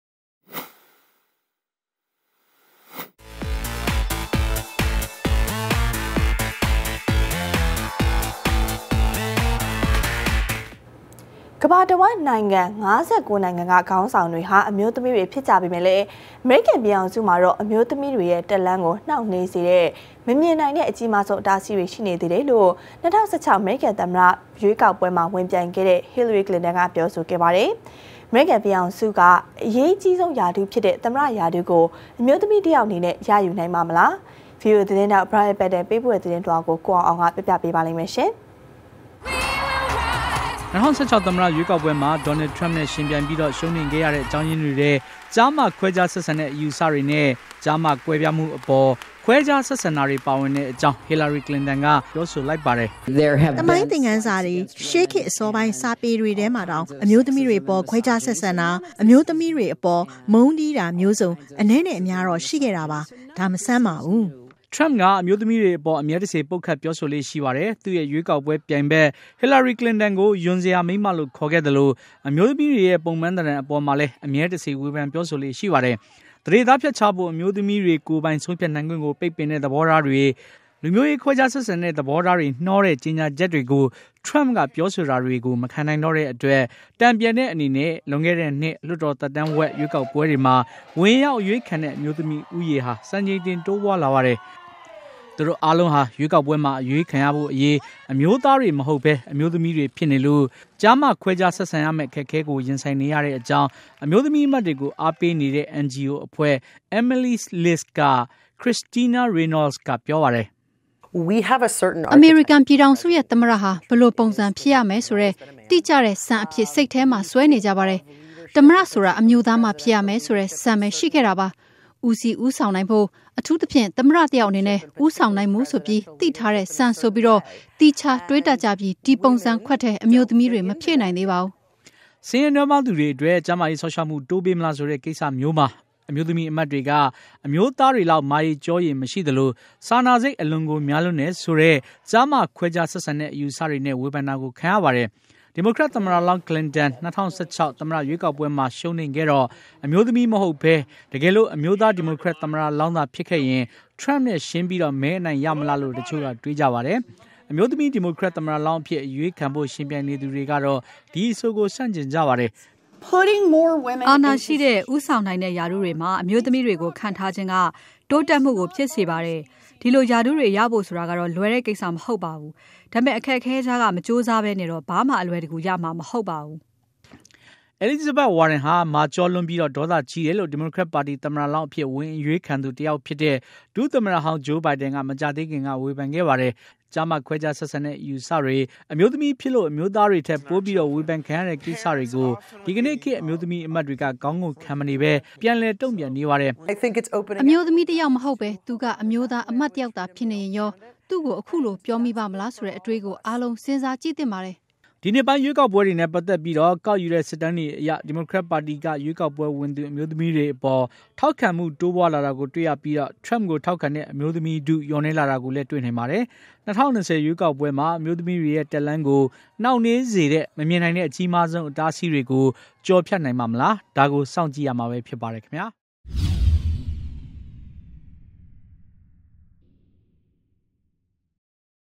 အမေရိကန်သမ္မတရွေးကောက်ပွဲနဲ့ အမျိုးသမီးများ
MC: ကမ္ဘာတဝှမ်း နိုင်ငံ ၅၉ နိုင်ငံက ခေါင်းဆောင်တွေဟာ အမျိုးသမီးတွေ ဖြစ်ကြပေမယ့် အမေရိကန်ပြည်ထောင်စုမှာတော့ အမျိုးသမီးတွေရဲ့ တက်လမ်းကို နှောင့်နှေးစေတဲ့ မမြင်နိုင်တဲ့ အကြီးမားဆုံး အတားအဆီးတွေ ရှိနေသေးတယ်လို့ ၂၀၁၆ အမေရိကန်သမ္မတအဖြစ် ဝင်အရွေးခံခဲ့တဲ့ Hillary Clinton က ပြောခဲ့ပါတယ်။ အမေရိကန်ပြည်ထောင်စုမှာ အရေးအကြီးဆုံး ရာထူးဖြစ်တဲ့ သမ္မတရာထူးကို အမျိုးသမီးတယောက်က ရယူနိုင်ပါ့မလား။ VOA သတင်းထောက် Brian Padden ပေးပို့တဲ့ သတင်းလွှာကို ကိုအောင်ဆွေက ပြောပြပေးပါမယ်။

( ZawGyi )

အေမရိကန္သမၼတေ႐ြးေကာက္ပြဲနဲ႔ အမ်ိဳးသမီးမ်ား

MC: ကမာၻတဝွမ္း ႏိုင္ငံ ၅၉ ႏိုင္ငံက ေခါင္းေဆာင္ေတြဟာ အမ်ိဳးသမီးေတြ ျဖစ္ၾကေပမယ့္ အေမရိကန္ျပည္ေထာင္စုမွာေတာ့ အမ်ိဳးသမီးေတြရဲ႕ တက္လမ္းကို ေႏွာင့္ေႏွးေစတဲ့ မျမင္ႏိုင္တဲ့ အႀကီးမားဆုံး အတားအဆီးေတြ ရွိေနေသးတယ္လို႔ ၂၀၁၆ အေမရိကန္သမၼတအျဖစ္ ဝင္အေ႐ြးခံခဲ့တဲ့ Hillary Clinton က ေျပာခဲ့ပါတယ္။ အေမရိကန္ျပည္ေထာင္စုမွာ အေရးအႀကီးဆုံး ရာထူးျဖစ္တဲ့ သမၼတရာထူးကို အမ်ိဳးသမီးတေယာက္က ရယူႏိုင္ပါ့မလား။ VOA သတင္းေထာက္ Brian Padden ေပးပို႔တဲ့ သတင္းလႊာကို ကိုေအာင္ေဆြက ေျပာျပေပးပါမယ္။